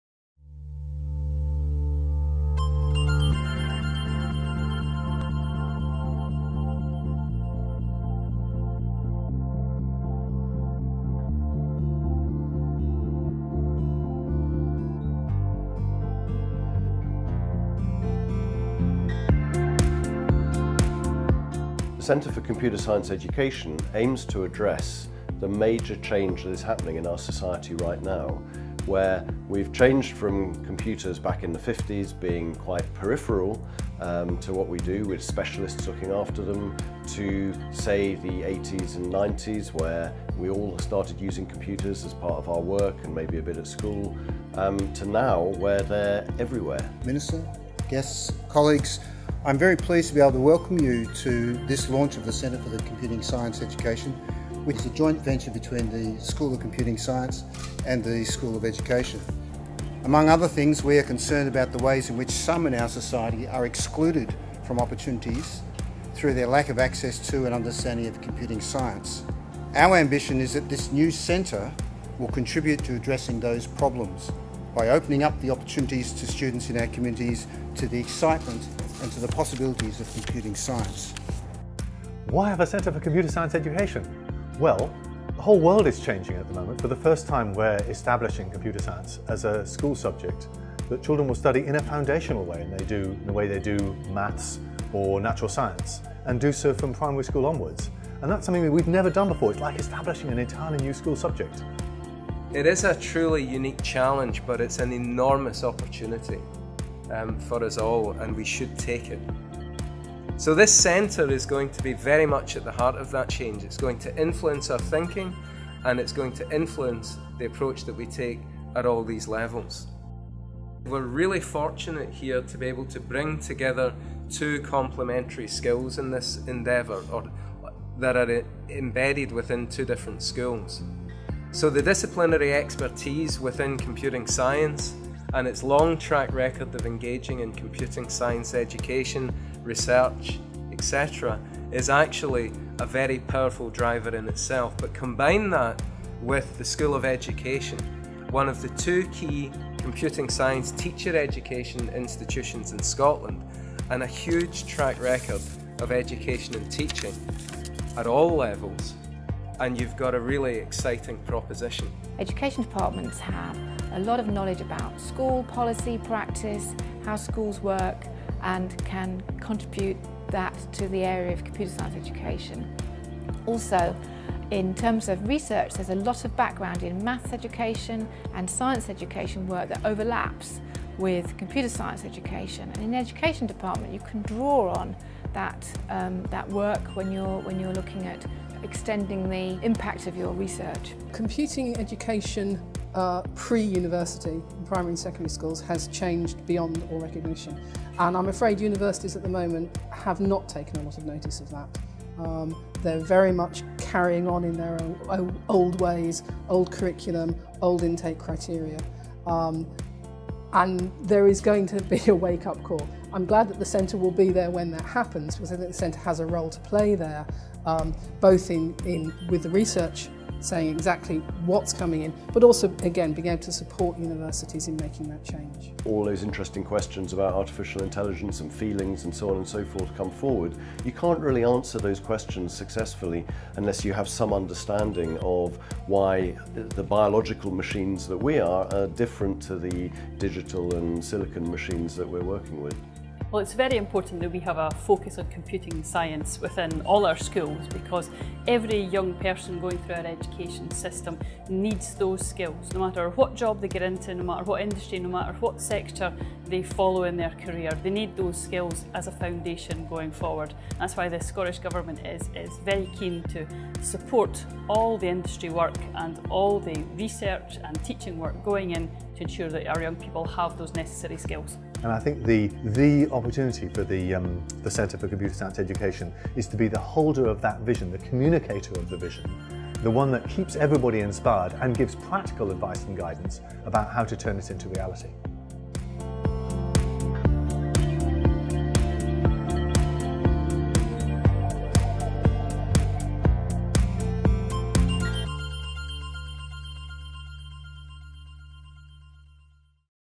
Centre for Computing Science Education (CCSE)
Filmed at its launch on 1st September 2017, this short video provides an overview on the importance of the Centre for Computing Science Education in facilitating improved research and leadership in computer science education as it develops as a mainstream school discipline.